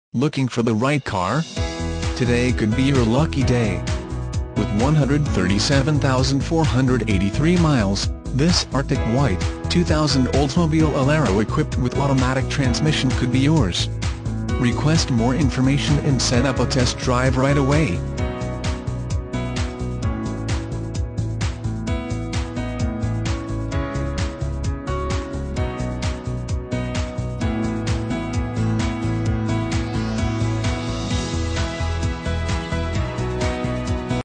2000 OLDSMOBILE ALERO White Bear Lake, MN 12153
2000 OLDSMOBILE ALERO White Bear Lake, MN
Stock #12153

1801 E County Road F

Looking for the right car? Today could be your lucky day. With 137483 miles, this Arctic White, 2000 Oldsmobile Alero equipped with automatic transmission could be yours. Request more information and set up a test drive right away.